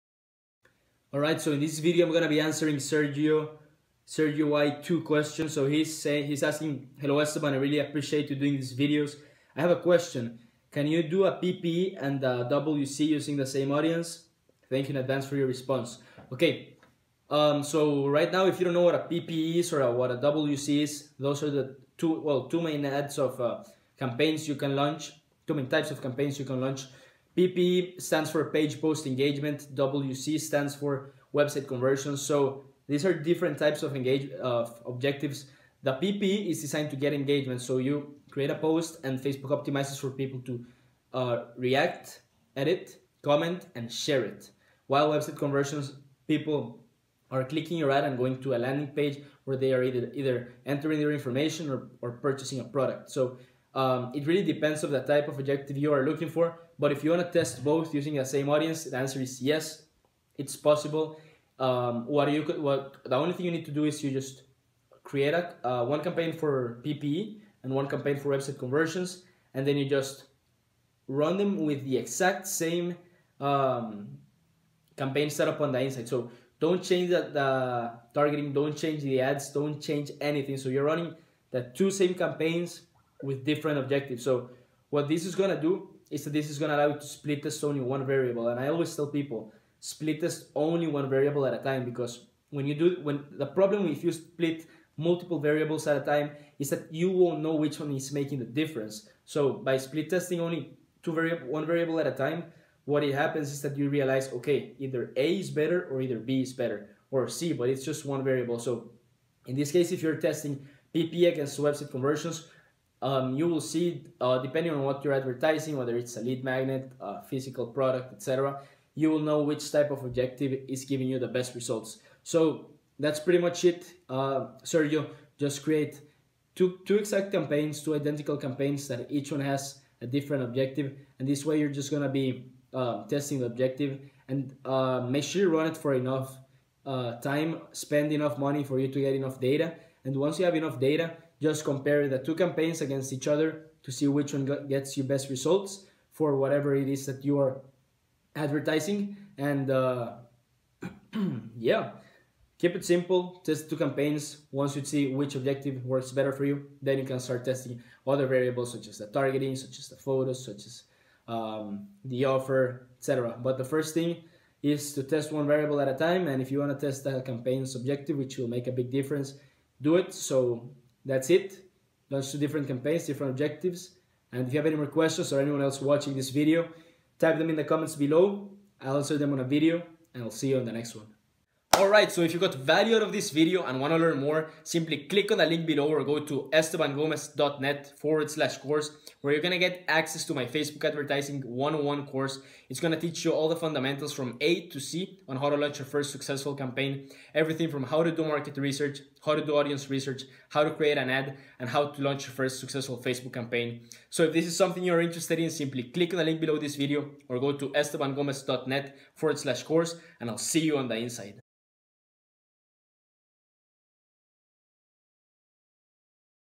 How To Run Multiple Facebook Ad Campaigns Targeting The Same Audience | AskEstebanGomez #63
In this episode I will explain how to run two different Facebook advertising campaign objectives (Website Conversion vs PPE) targeting the same audience.

In this episode I talk